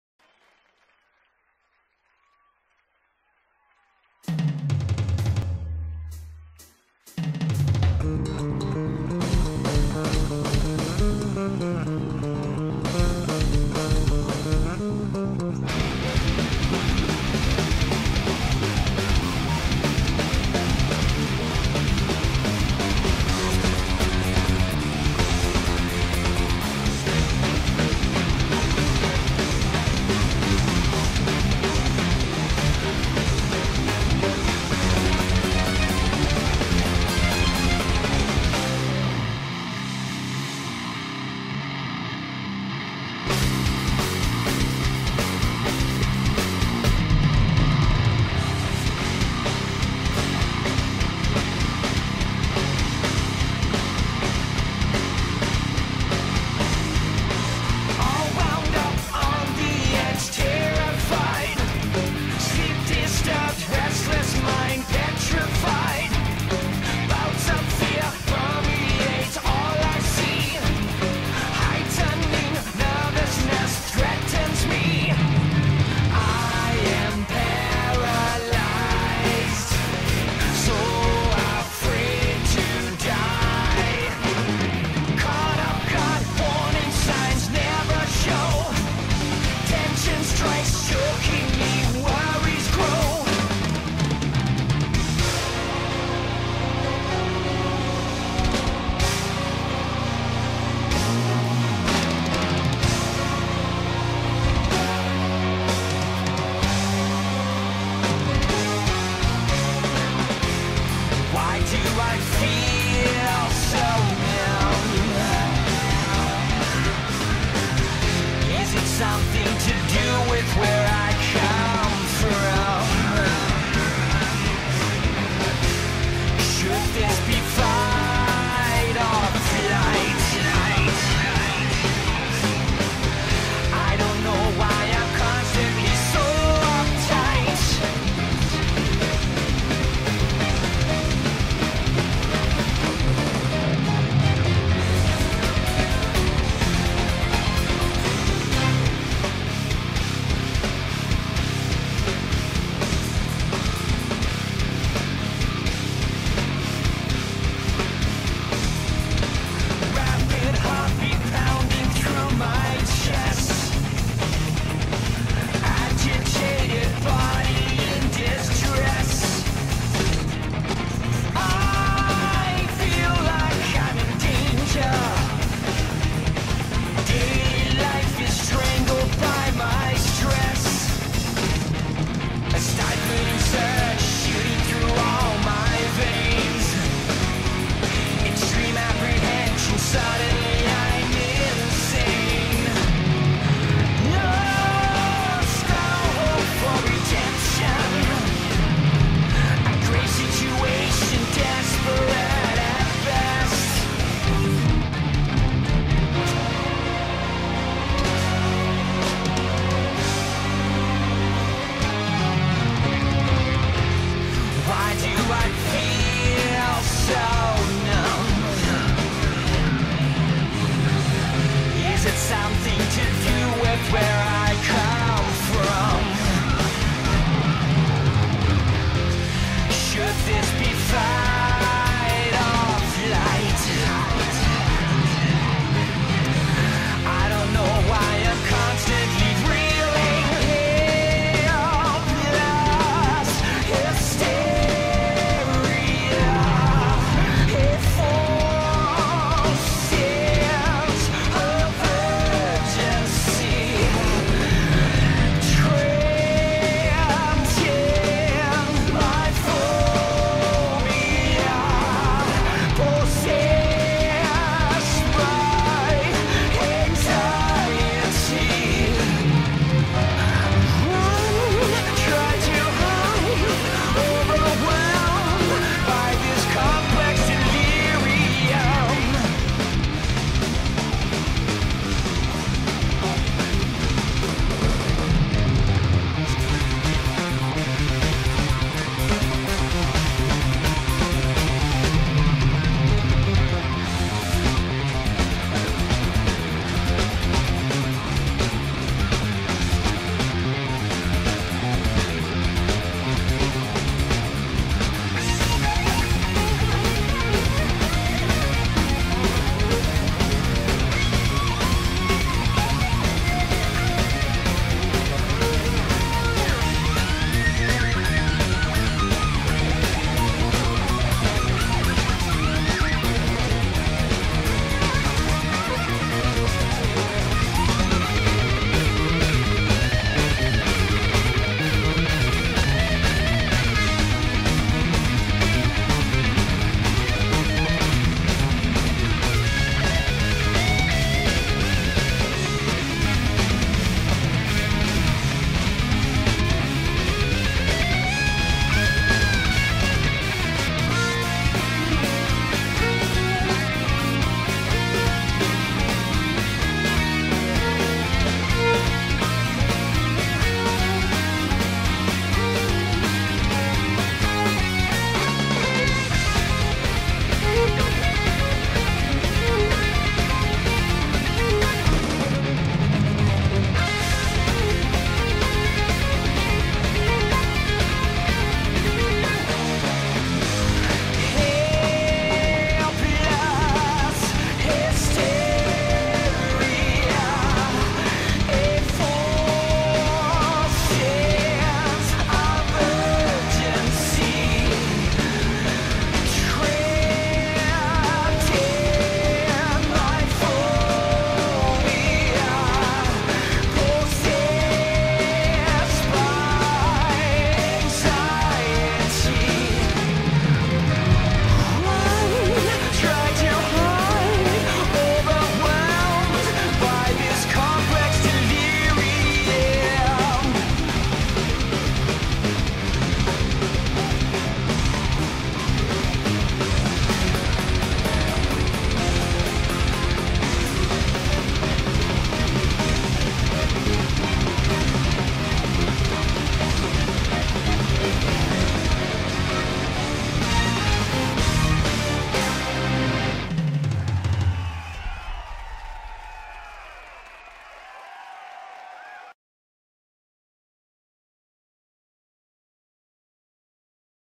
Panic Attack (HD PVR Test)
Testing quality of Hauppauge HD PVR

Not a good run, simply testing.

Captured in 720p MT2S - Converted to WMV - Encoded with Dr. Divx